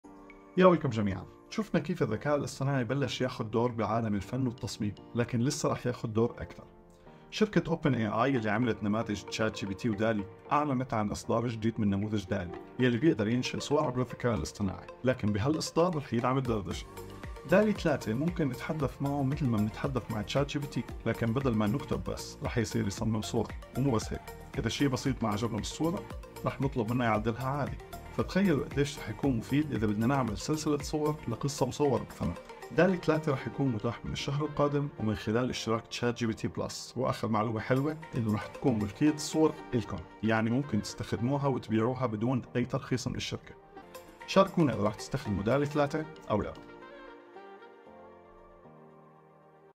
نموذج دالي لتصميم الصور صار يدعم الدردشة .. كأنو عم ندردش مع ديزاينر 😎
ديزاينر ما رح يمل من طلباتكن 😁